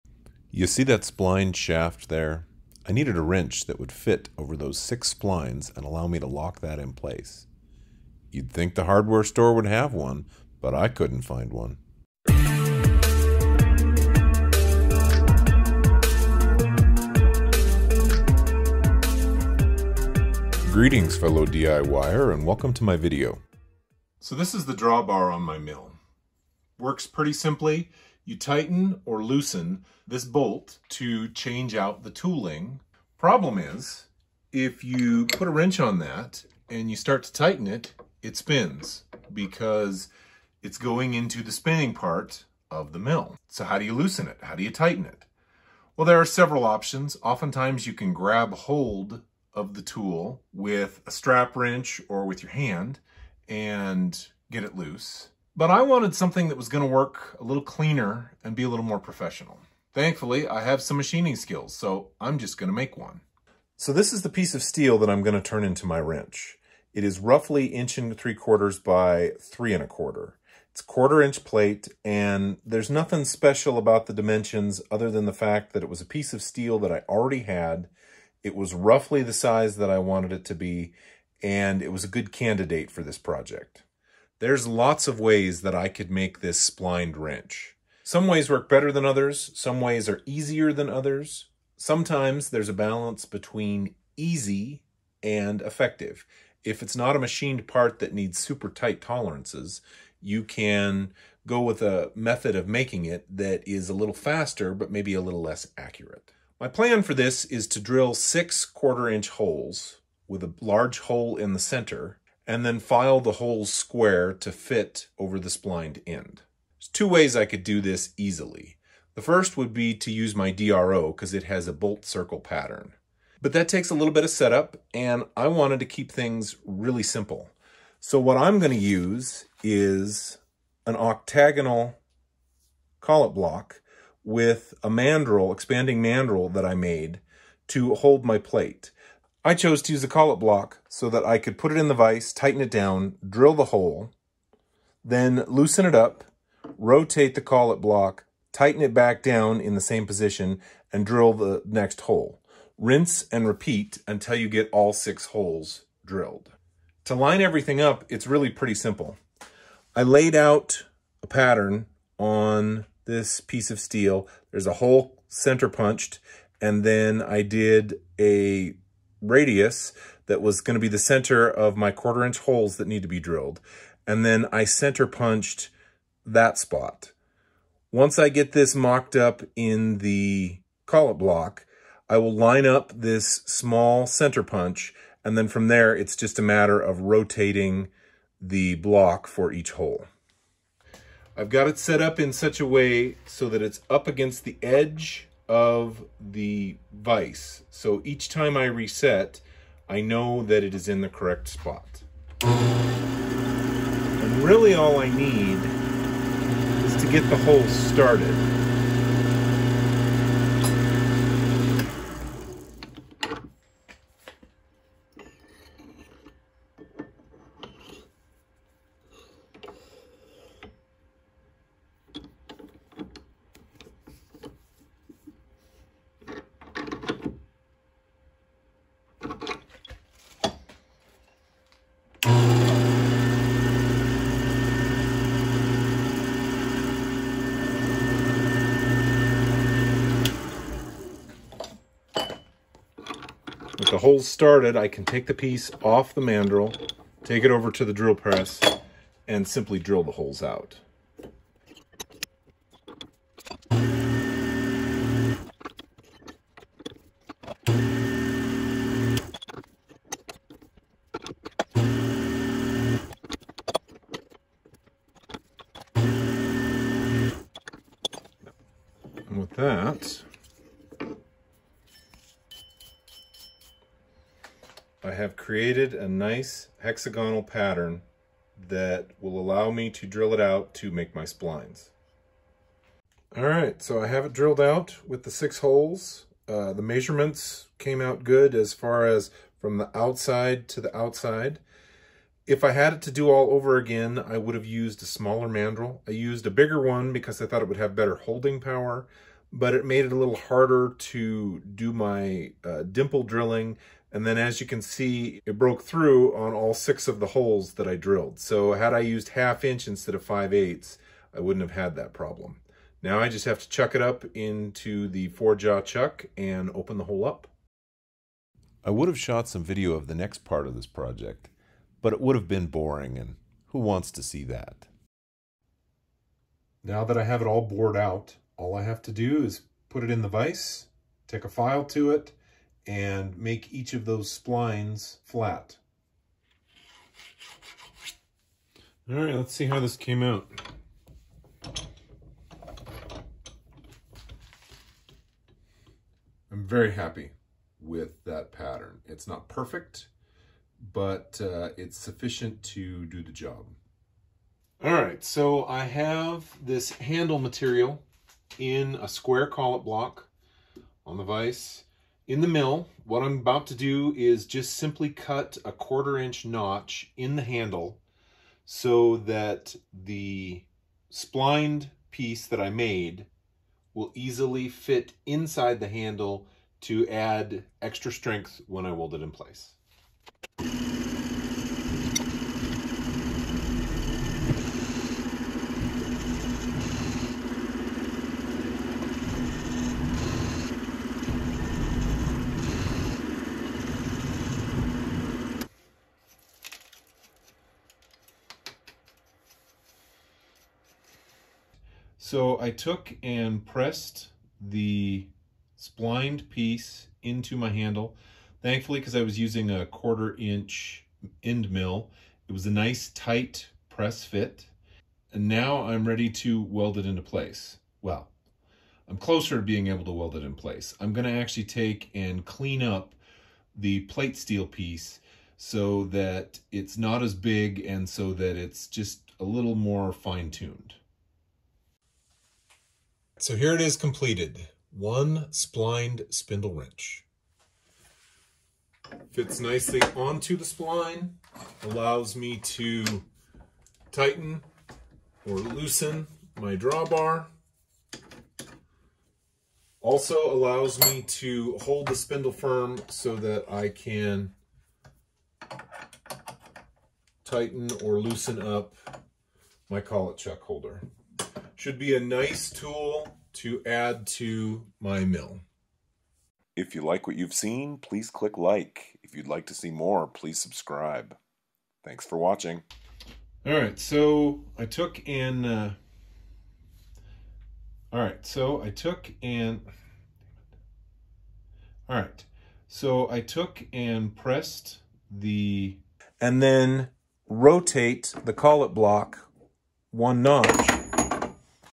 Making a DIY Internal Splined Spindle Wrench Without a Keyway Broach for my 3-in-1 type mill
I wanted a way to secure the spindle on my mill when tightening/loosening the draw bar bolt and when tightening/loosening the collet chuck.  The mill spindle is a nice 6 splined shaft and a splined wrench seamed like the best way to go.  I do not have a keyway broach so I made the part by drilling out the basic shape and then doing some file work to get it to fit.